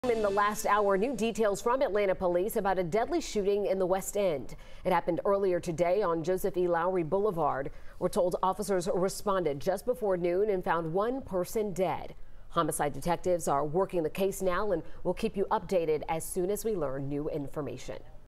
Detectives investigating deadly shooting in Atlanta's west end
1 person was shot dead, according to Atlanta Police Department investigators.